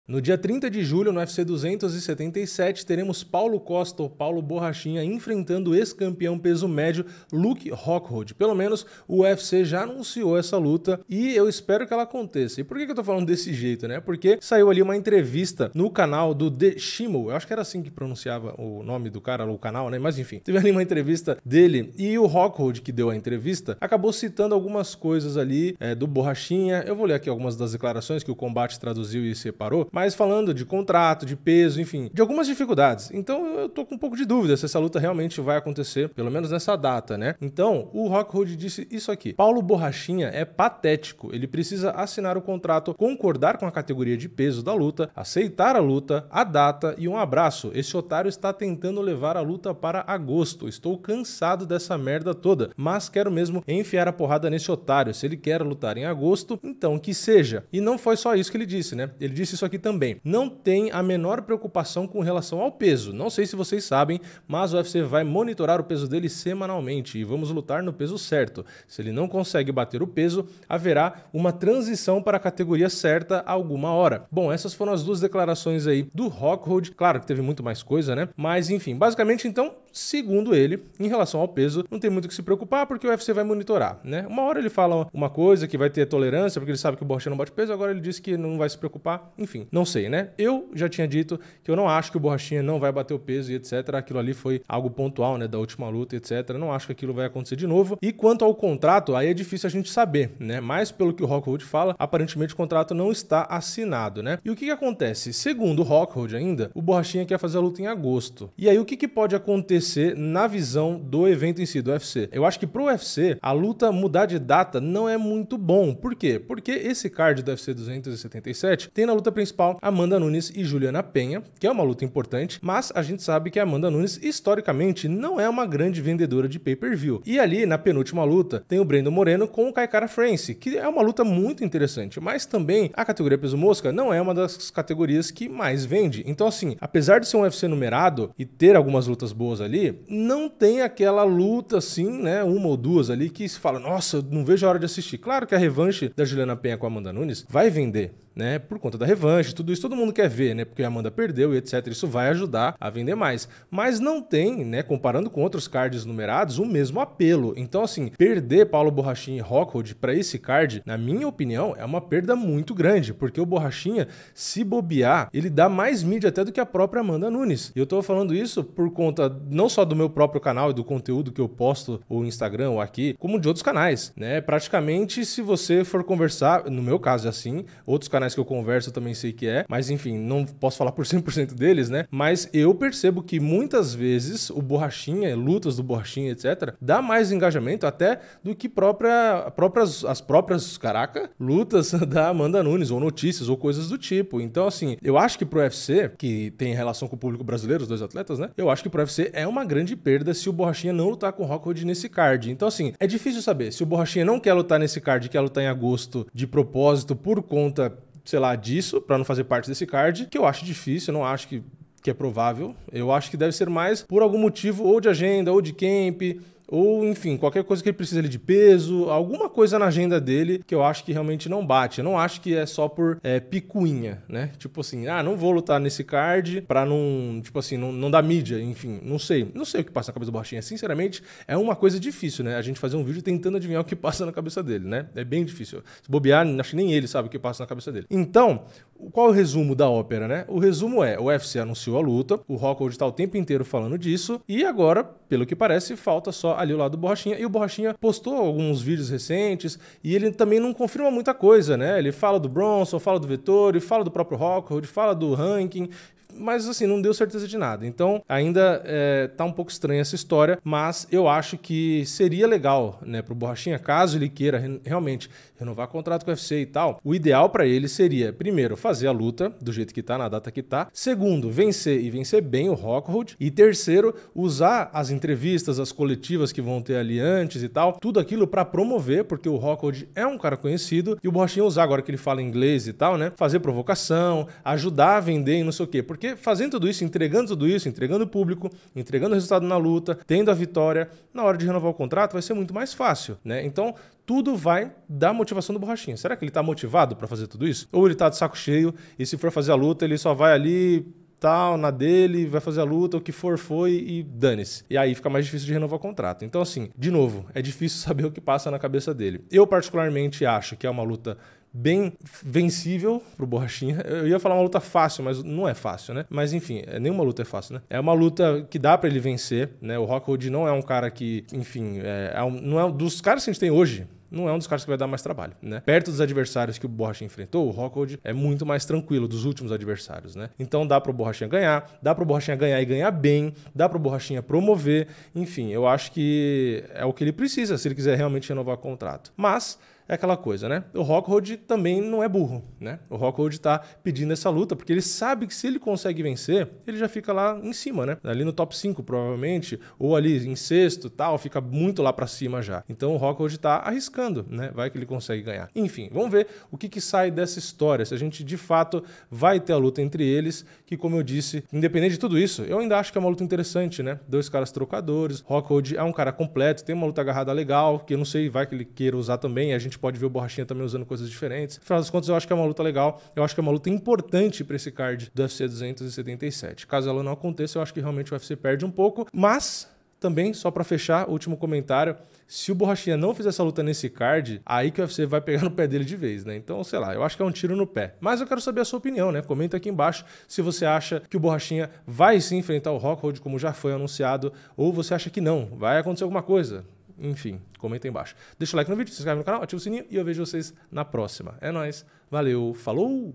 BORRACHINHA XINGADO E ACUSADO DE QUERER ADIAR A LUTA!
Luke Rockhold atacou Paulo Borrachinha mais uma vez!